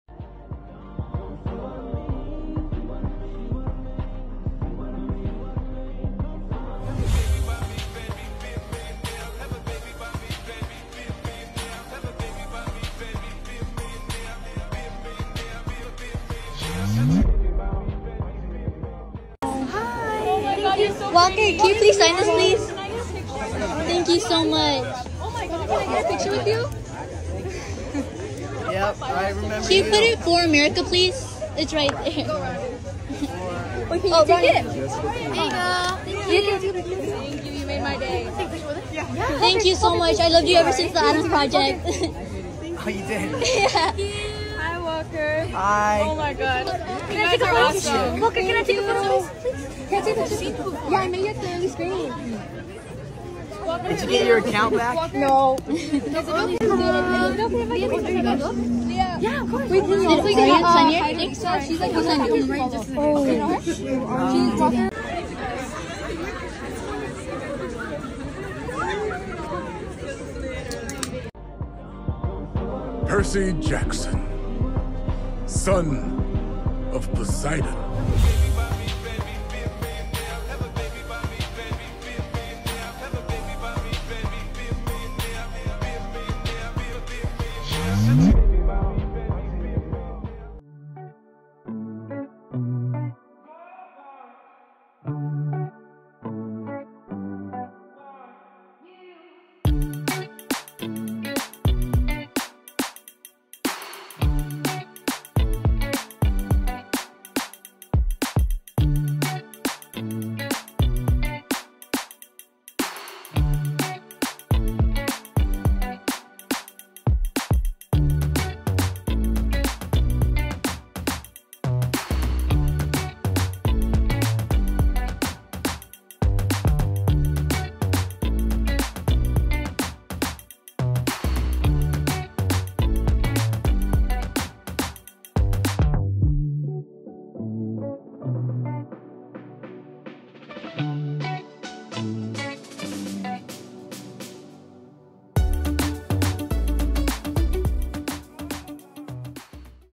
Walker Scobell at Santa Monica Pier | Percy Jackson and the Olympians ⚡
Walker Scobell with his co-stars Aryan Simhadri and Leah Sava Jeffries for the Percy Jackson and the Olympians Meet & Greet event at Santa Monica pier on Jan 29, 2024 ⚡

Clips from fans and media posted on Instagram, Twitter, and TikTok. Credit to original video owners.